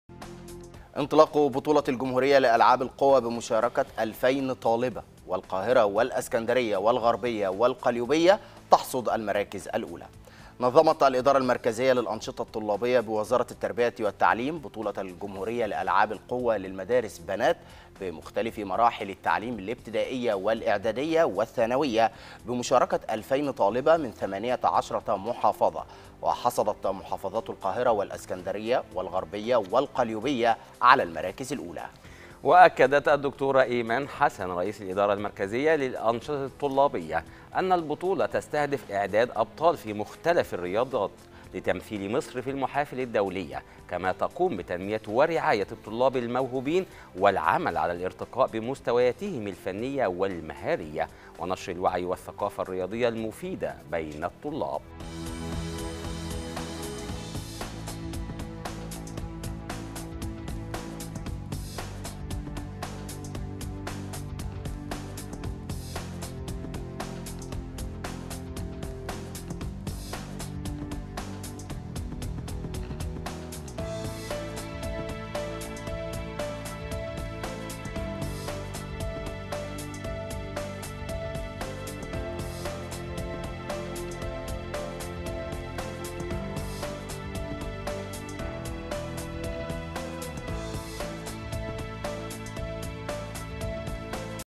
انطلاق بطولة الجمهورية لألعاب القوى بمشاركة ٢٠٠٠ طالبة
انطلاق بطولة الجمهورية لألعاب القوى بمشاركة ٢٠٠٠ طالبة.. والقاهرة والإسكندرية والغربية والقليوبية تحصد المراكز الأولى